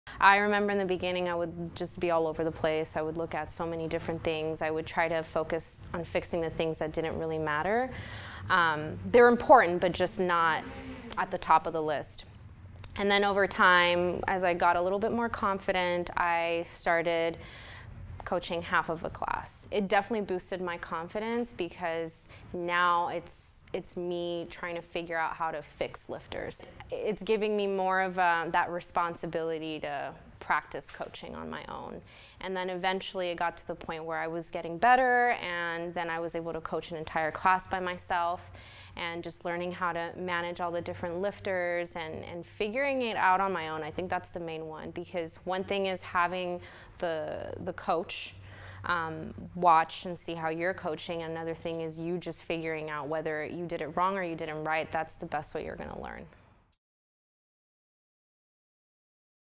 Jules On Her Starting Strength Coach Apprenticeship
Coach Jules Gonzalez explains how her Starting Strength Coach apprenticeship at The Strength Co. has helped her on her path to becoming a certified SSC.